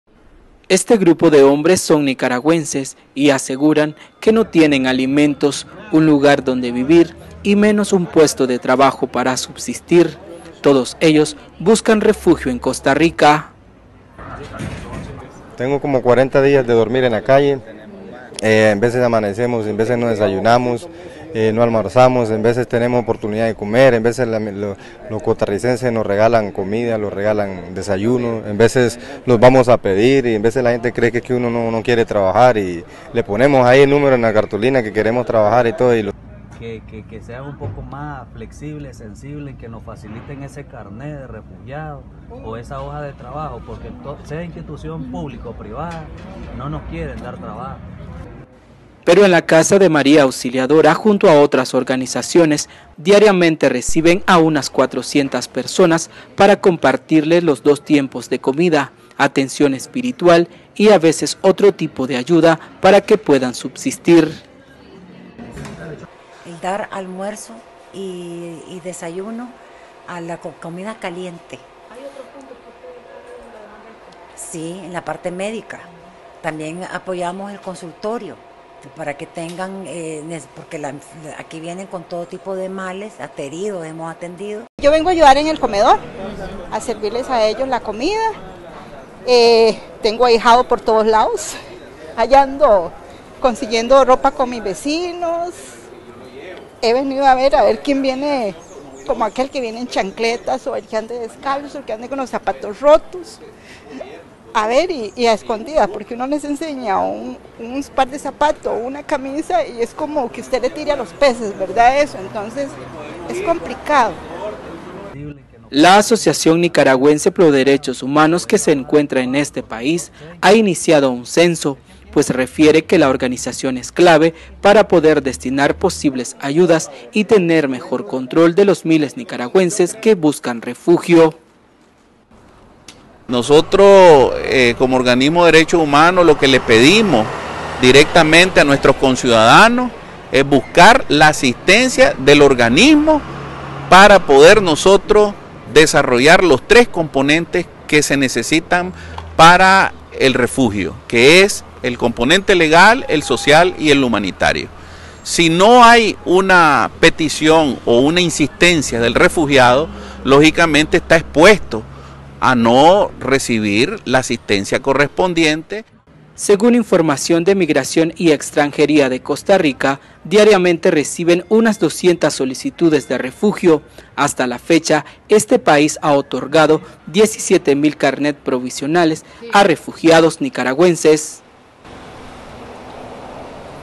Organizaciones sociales y de DD.HH. brindan ayuda a nicaragüenses en Costa Rica
Son las 12:45 del medio día. Un grupo de al menos 15 hombres originarios de Nicaragua, están a las afueras de la Casa de María Auxiliadora, a unas cinco cuadras del famoso Parque de La Merced, en la capital costarricense. Ahí, como parte de la obra social Sor María Romero, se comparten alimentos a los nicaragüenses que buscan refugio por la crisis de su país.